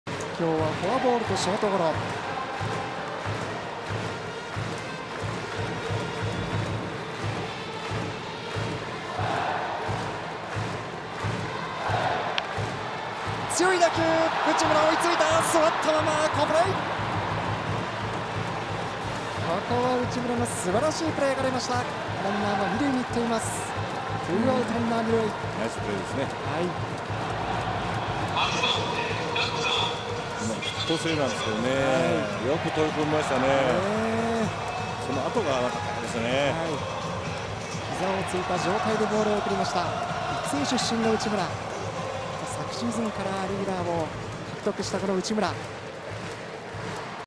【好プレー】内村、体勢崩しながら粘り強い守備 5月3日 西武-楽天
楽天・内村がヒット性の当たりを横っ跳びで押さえ、体勢を崩しながらも一塁へ送球してアウトを奪った。（5月3日 西武-楽天戦）